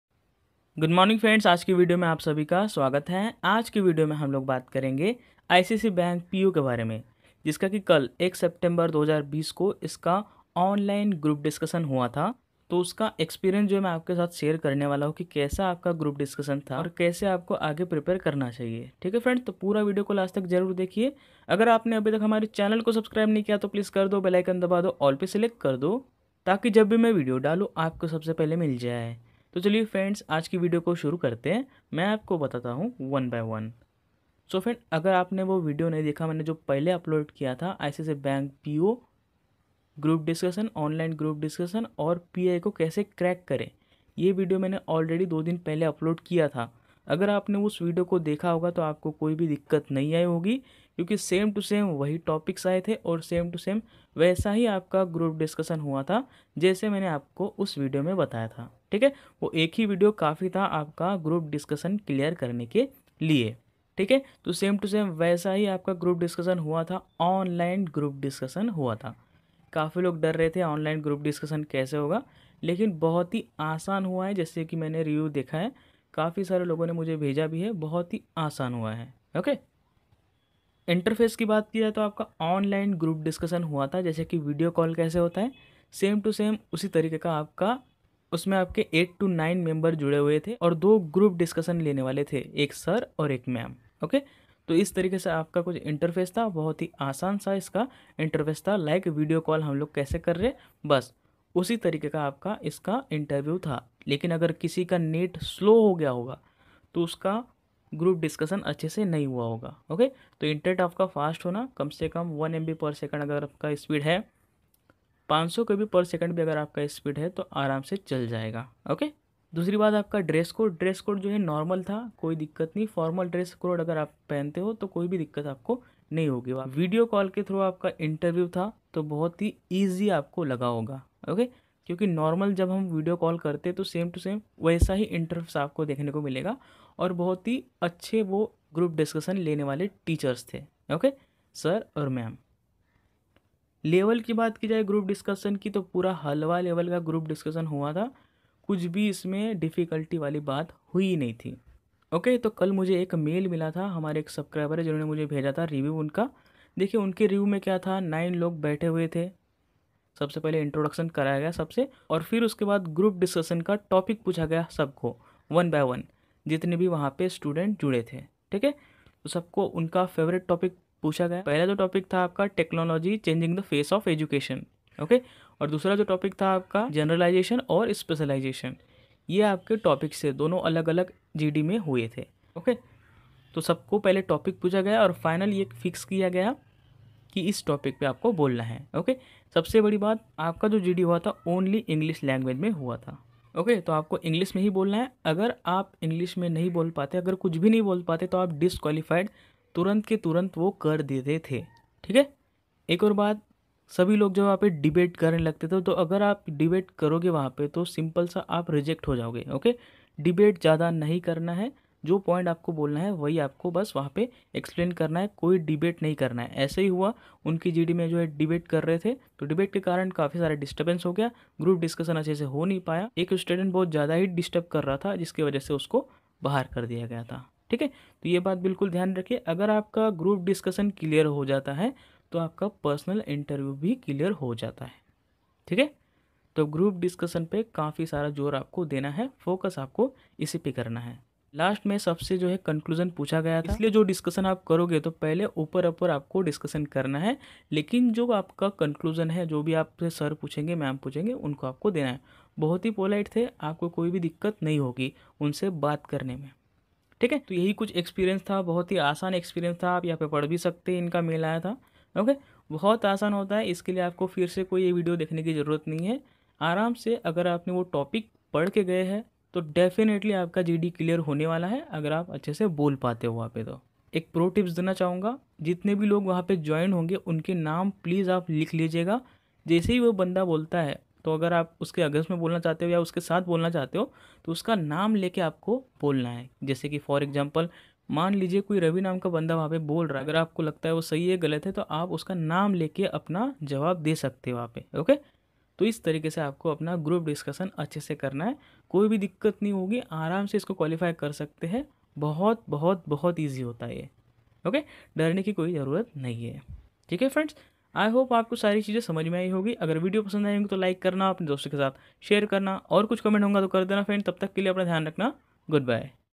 ICICI Bank PO 2020 Online GD Experience | ICICI Bank PO 2020 Online Group Discussion Tips
In this video I have discussed about the ICICI Bank PO 2020 Online GD Experience.How the group discussion was done .Watch full video to know more about the ICICI Bank PO Online Group Discussion.

Telegram Group Name :-@youngerage73